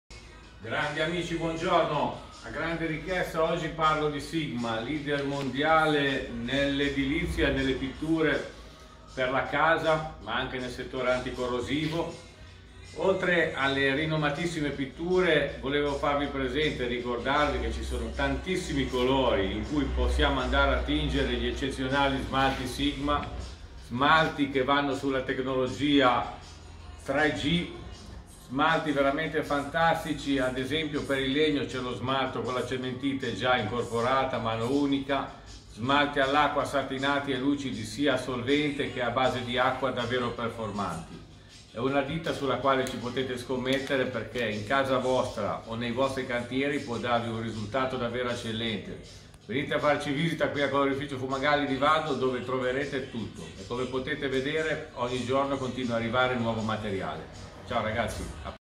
colorificio Fumagalli vado ligure e gli smalti Top Sigma Coatings
eccezionali; a base di acqua e/o solvente; coloratissimi; resistenti ai graffi; alle abrasioni; resistenti allo sporco e all unto. tooop!